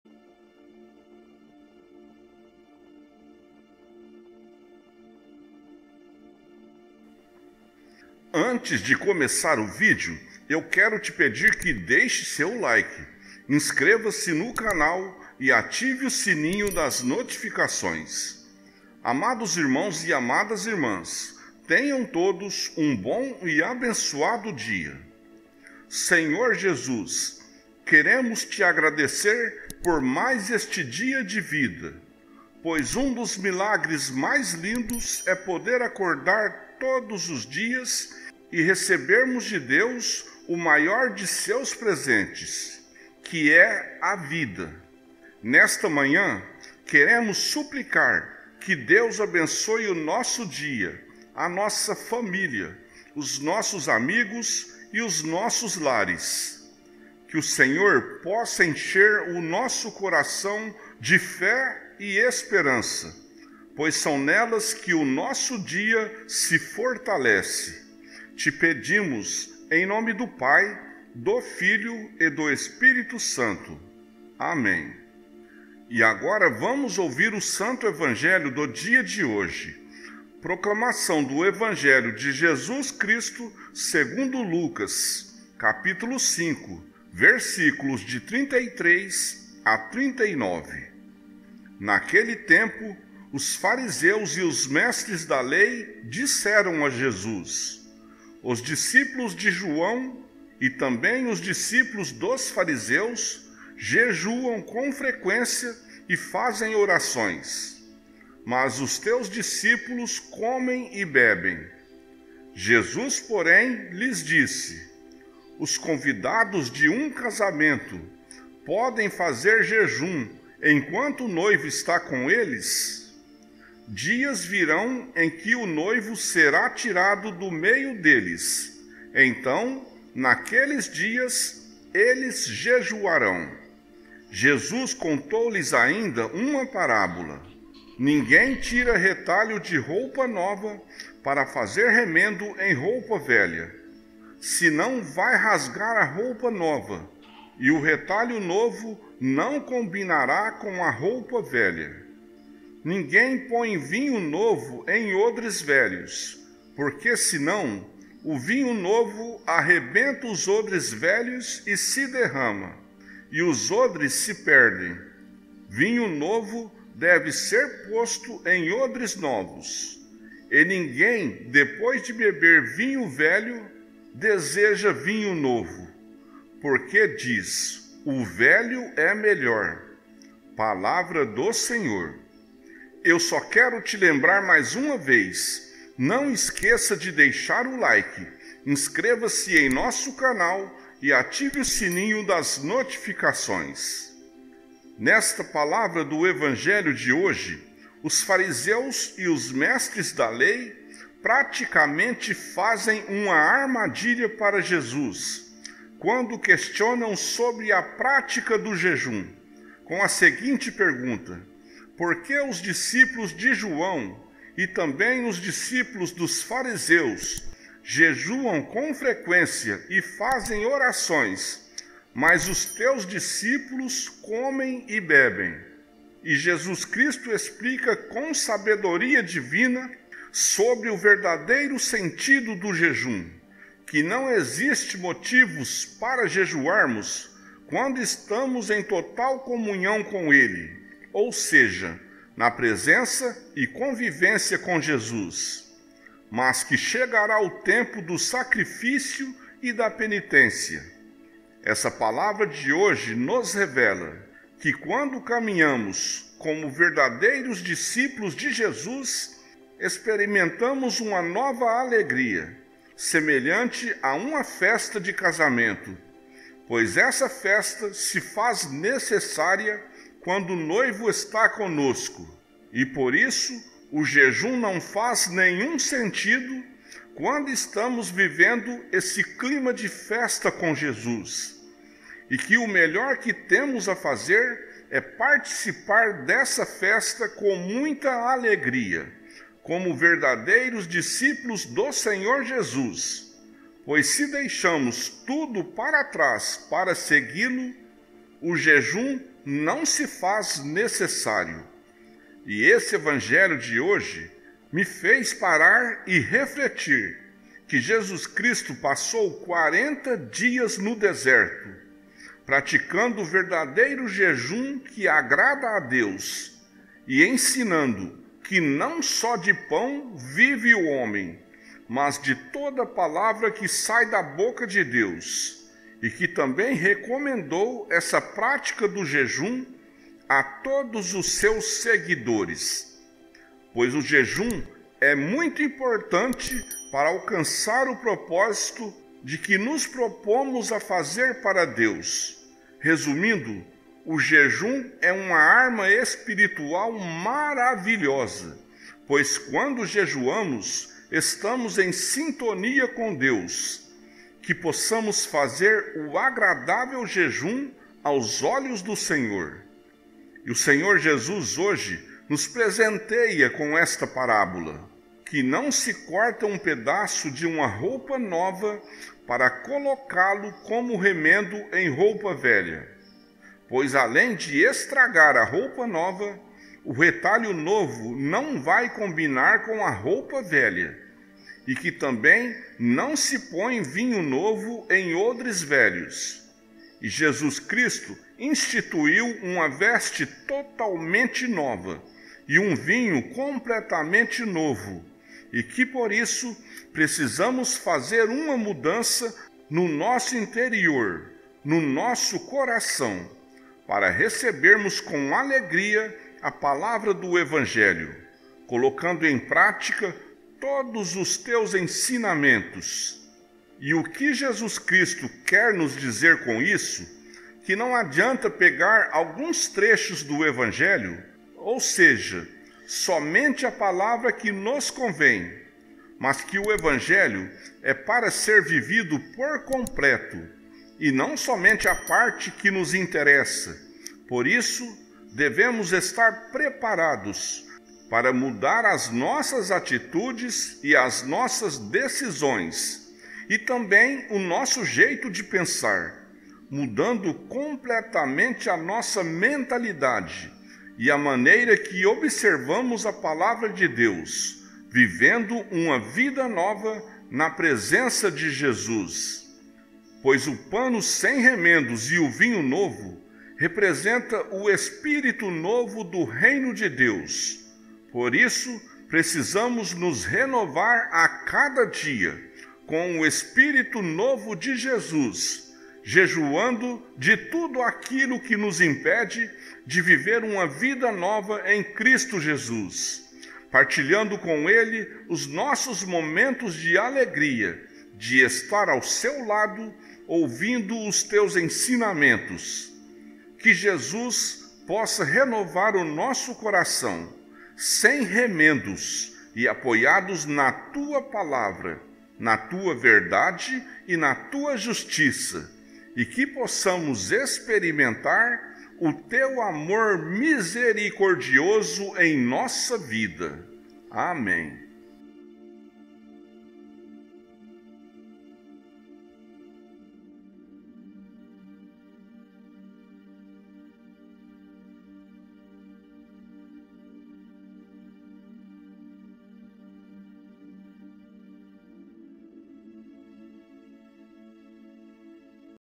EVANGELHO-DO-DIA-DE-HOJE-02-10 | (LC-5,33-39) | ORAÇÃO-DE-HOJE
O santo evangelho do dia de hoje é de São Lucas, onde o Senhor Jesus nos ensina o verdadeiro sentido do Jejum.

E na oração de hoje desta manhã, é uma oração de cura, onde suplicaremos ao Senhor um coração cheio de fé e esperança, pois são nelas que o nosso dia se fortalece.

Que possamos fazer a nossa oração da manhã de hoje, ouvir o evangelho de hoje e também a reflexão do evangelho com os ouvidos do coração.

Proclamação do Evangelho de Jesus Cristo segundo Lucas (Lc 5,33-39)

Naquele tempo,

os fariseus e os mestres da Lei disseram a Jesus:

Os discípulos de João,

e também os discípulos dos fariseus,

jejuam com frequência e fazem orações.

Mas os teus discípulos comem e bebem.

Jesus, porém, lhes disse:

Os convidados de um casamento

podem fazer jejum enquanto o noivo está com eles?

Dias virão em que o noivo será tirado do meio deles.

Então, naqueles dias, eles jejuarão.

Jesus contou-lhes ainda uma parábola:

Ninguém tira retalho de roupa nova

para fazer remendo em roupa velha;

senão vai rasgar a roupa nova,

e o retalho novo não combinará com a roupa velha.

Ninguém põe vinho novo em odres velhos;

porque, senão,

o vinho novo arrebenta os odres velhos e se derrama;

e os odres se perdem.

Vinho novo deve ser posto em odres novos.

E ninguém, depois de beber vinho velho,

deseja vinho novo; porque diz: o velho é melhor”.

Nesta palavra do evangelho de hoje,

os fariseus e os mestres da lei,

praticamente fazem uma armadilha para Jesus,

quando questionam sobre a pratica do jejum,

com a seguinte pergunta:

porque os discípulos de João,

Essa palavra de hoje nos revela,

que quando caminhamos

como verdadeiros discípulos de Jesus,

experimentamos uma nova alegria,

semelhante a uma festa de casamento,

pois essa festa se faz necessária,

quando o noivo está conosco.

E por isso, o jejum não faz nenhum sentido,

quando estamos vivendo esse clima de festa com Jesus,

e que o melhor que temos a fazer,

é participar dessa festa com muita alegria,

como verdadeiros discípulos do Senhor Jesus.

pois se deixamos tudo para trás para segui-lo,

o jejum não se faz necessário.

E esse evangelho de hoje me fez parar e refletir,

que Jesus Cristo passou 40 dias no deserto,

praticando o verdadeiro jejum que agrada a Deus,

e ensinando que não só de pão vive o homem,

mas de toda palavra que sai da boca de Deus.

e que também recomendou essa prática do jejum,

a todos os seus seguidores.

Pois o jejum é muito importante, para alcançar o propósito

do que nos propomos a fazer para Deus.

Resumindo, o jejum é uma arma espiritual maravilhosa,

Pois quando jejuamos, estamos em sintonia com Deus.

Que possamos fazer o agradável jejum aos olhos do Senhor.

E o Senhor Jesus hoje nos presenteia com esta parábola,

que não se corta um pedaço de uma roupa nova

para colocá-lo como remendo em roupa velha.

pois além de estragar a roupa nova,

o retalho novo não vai combinar com a roupa velha,

e que também não se põe vinho novo em odres velhos,

e Jesus Cristo instituiu uma veste totalmente nova,

e um vinho completamente novo,

e que por isso, precisamos fazer uma mudança,

no nosso interior, no nosso coração,

para recebermos com alegria, a palavra do evangelho,

colocando em prática, todos os seus ensinamentos.

E o que Jesus Cristo quer nos dizer com isso,

que não adianta pegar alguns trechos do evangelho,

ou seja, somente a palavra que nos convém,

mas que o evangelho é para ser vivido por completo,

e não somente a parte que nos interessa,

por isso, devemos estar preparados,

para mudar as nossas atitudes e as nossas decisões,

e também o nosso jeito de pensar,

mudando completamente a nossa mentalidade,

e a maneira que observamos a palavra de Deus,

vivendo uma vida nova, na presença de Jesus.

Pois o pano sem remendos e o vinho novo,

representa o espírito novo do Reino de deus.

Por isso, precisamos nos renovar a cada dia,

com o Espírito novo de Jesus,

jejuando de tudo aquilo que nos impede,

de viver uma vida nova em Cristo Jesus,

partilhando com ELE os nossos momentos de alegria,

de estar ao seu lado, ouvindo os seus ensinamentos.

Que Jesus Cristo possa renovar o nosso coração,

sem remendos e apoiados na tua palavra,

na tua verdade e na tua justiça,

e que possamos experimentar,

o teu amor misericordioso em nossa vida.

AMÉM!

Índice do Vídeo:

00:00 - Introdução
00:18 - Oração do dia de hoje
01:15 - Evangelho do dia de hoje segundo Lucas (Lc 5,33-39)
03:14 - Reflexão do evangelho de hoje